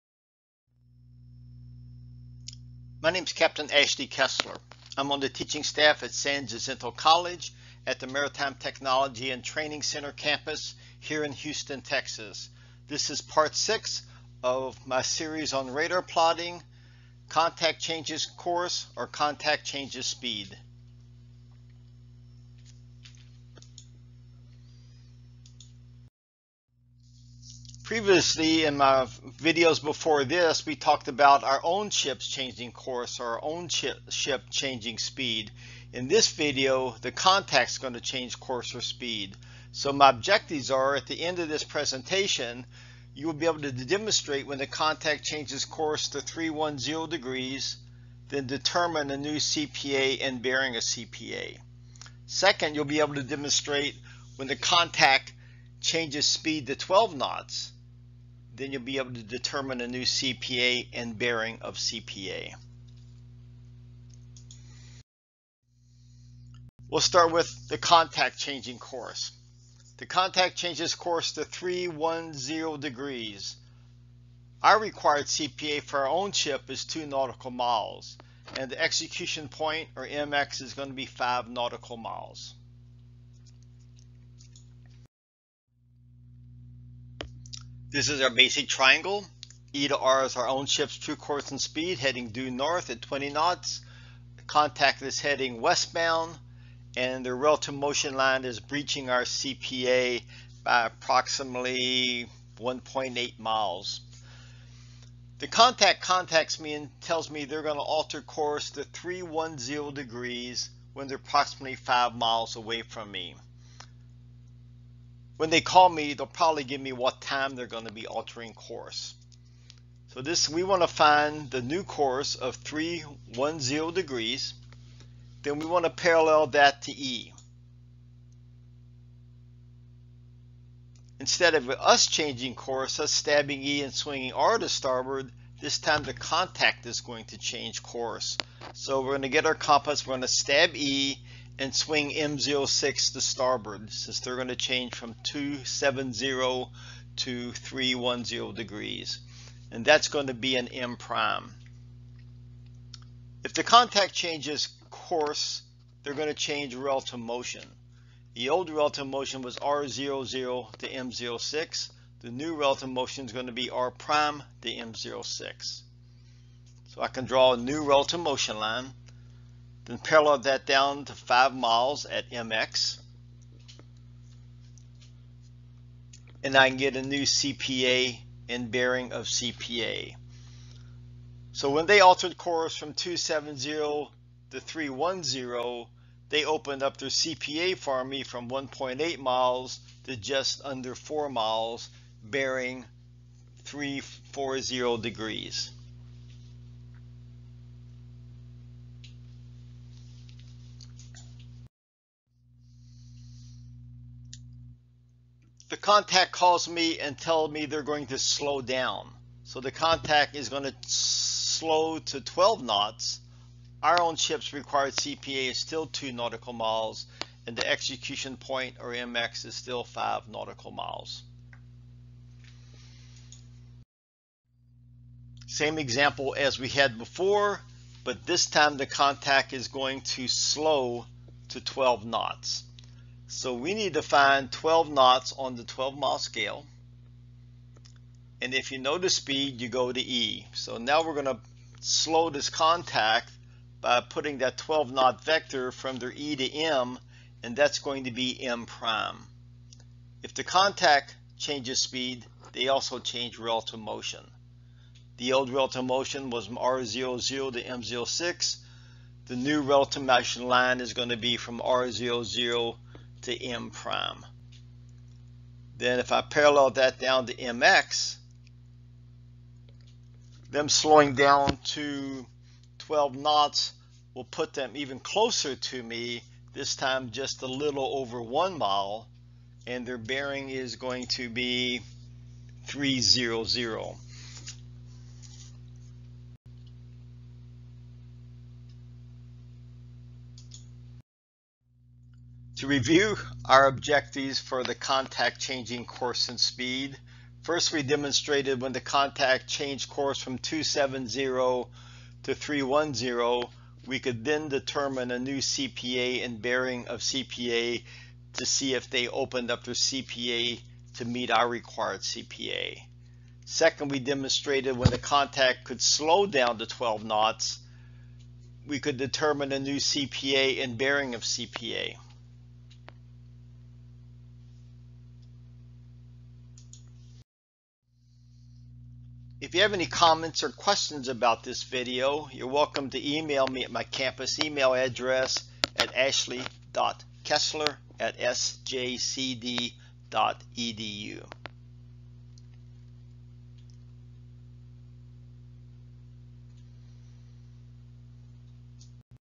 6. RADAR Plotting: Part Six - Contact Changes Course or Speed
Objectives:
1. Demonstrate when Contact Changes Course to 310⁰. Determine new CPA and Bearing of CPA.
2. Demonstrate when Contact Changes Speed to 12 knots. Determine new CPA and Bearing of CPA.